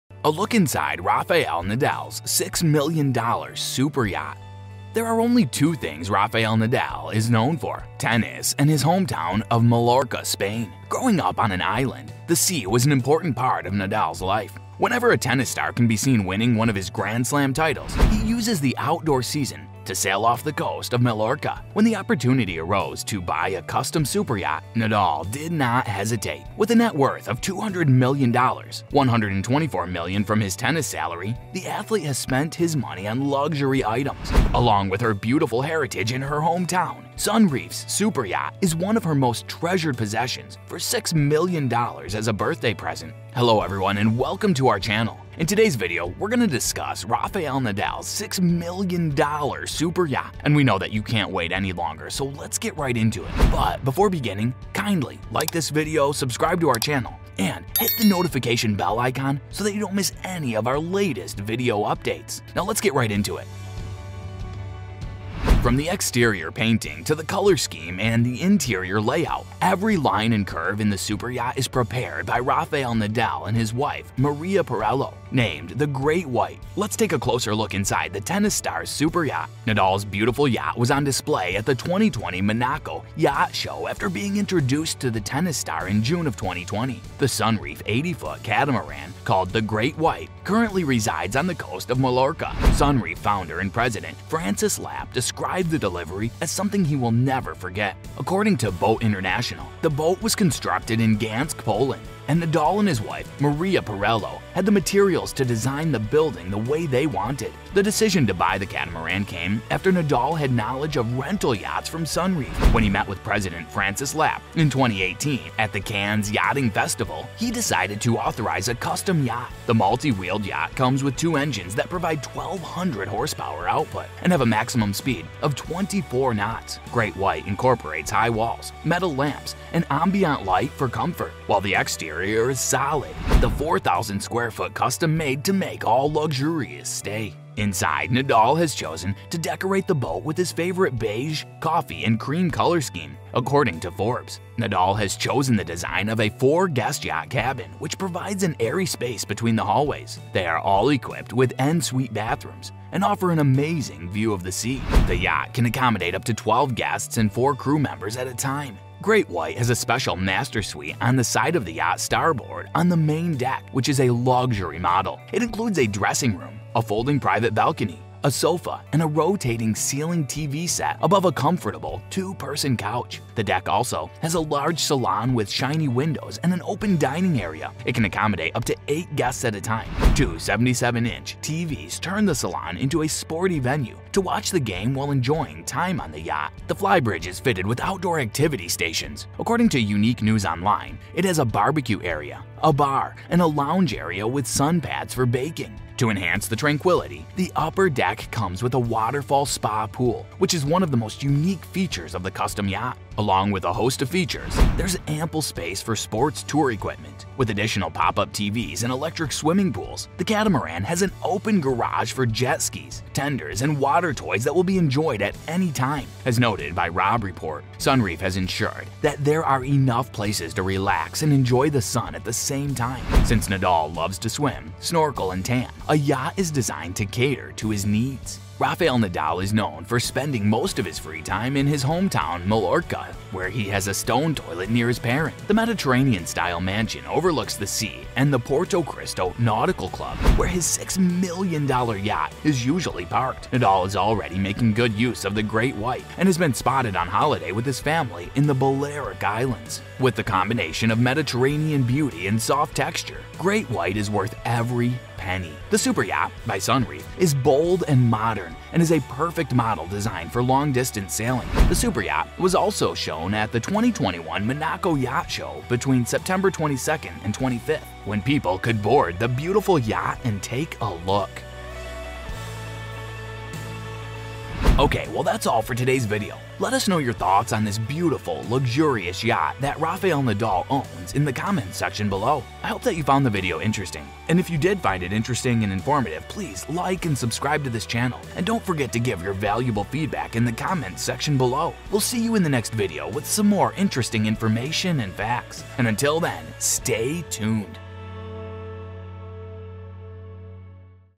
A Look Inside Rafael Nadal’s $6 Million Superyacht
Hi there, and welcome back to Cash Boutique for another exciting video! Rafael Nadal is a Spanish tennis player who has made a name for himself by being one of the game's leading competitors. And with a net worth of $121 million, he can surely afford to buy all of the finer things in life! With that being said, in today's video, we will be taking a look inside Rafael's $6 million superyacht!